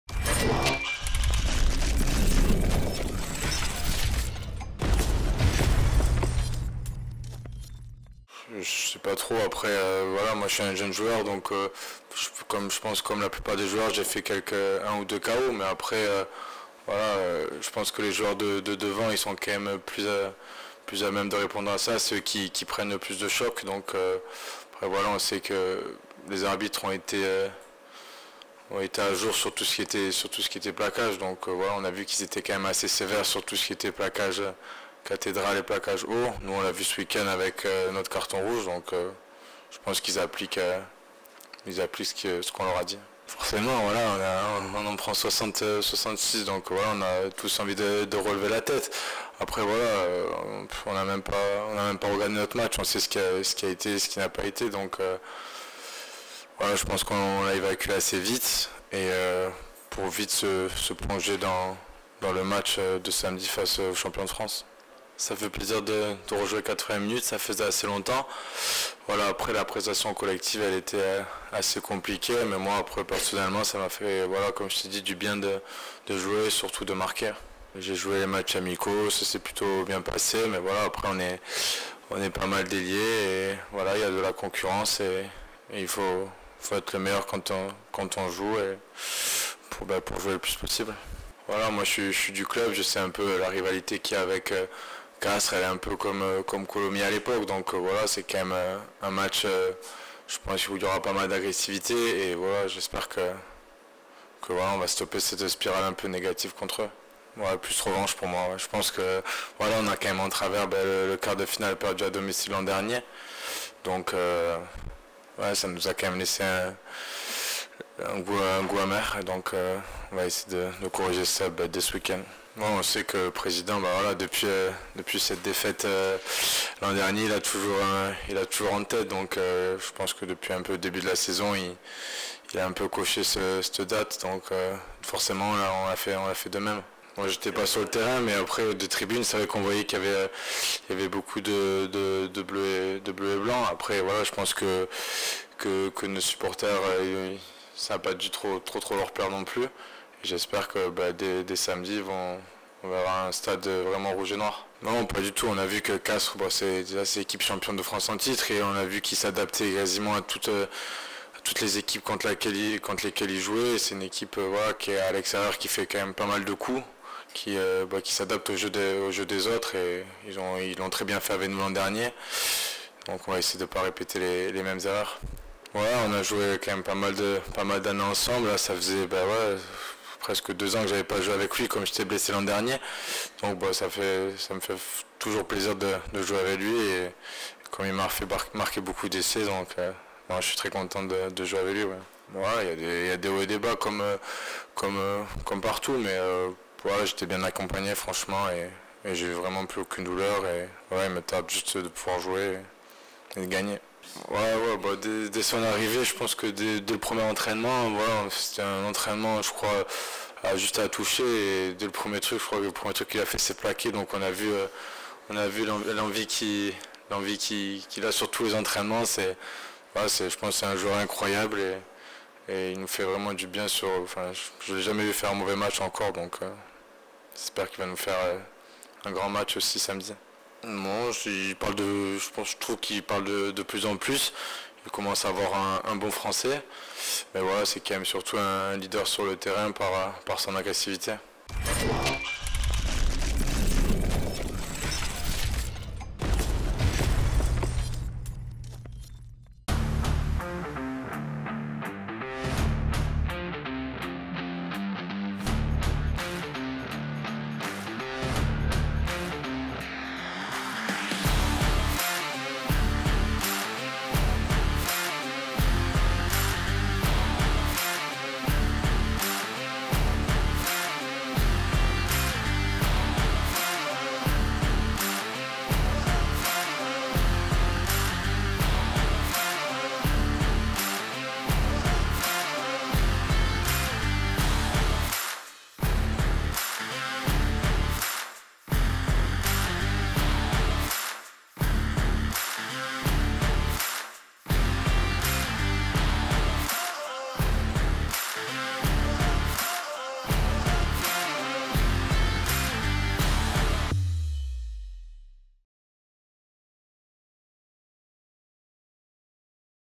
Stade-Castres : Interview d'Arthur Bonneval
L'ailier rouge et noir, Arthur Bonneval, revient sur le début de saison des Stadistes et sur le match pas comme les autres qui les attend ce samedi face au Castres Olympique.